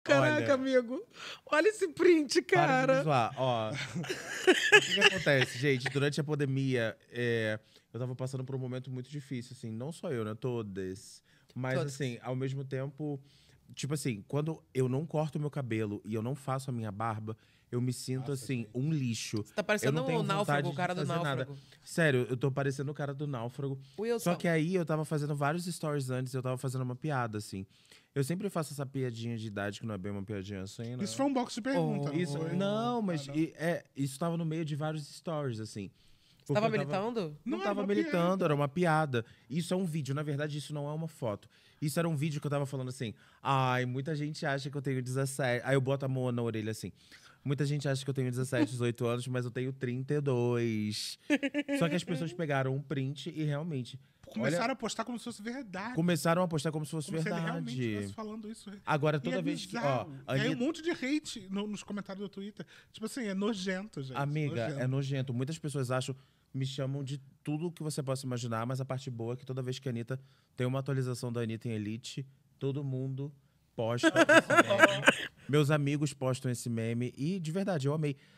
17 ANOS, CAIO REVELA? | Corte A Seita
Chega de especulações: Não, o @caiorevela não tem 17 anos!

Com quantos anos você descobriu a verdadeira idade do Caio? Comenta aqui!

Assista agora o episódio completo do videocast clicando no link da bio!

Amar seu corpo é um ato revolucionário! 🫂💞